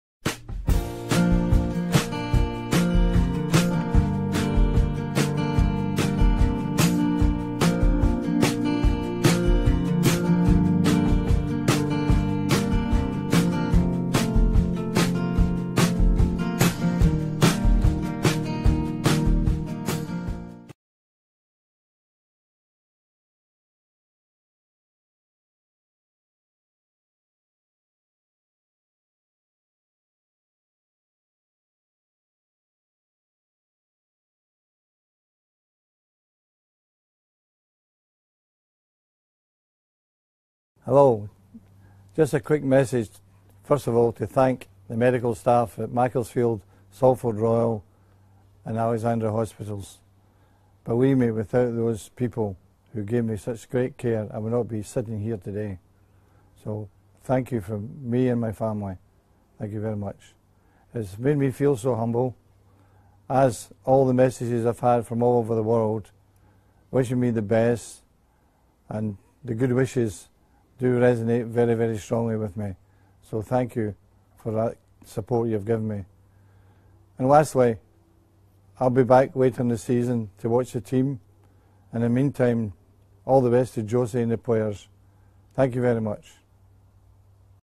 SIR ALEX FERGUSON SPEAK AFTER RECOVER FROM ILLNESS
Sir Alex Ferguson have thank all people in the World who prayed for him when he was at illness, where he suffered from brain hemorrhage which result into brain surgery. For now he doing well and fairly recover!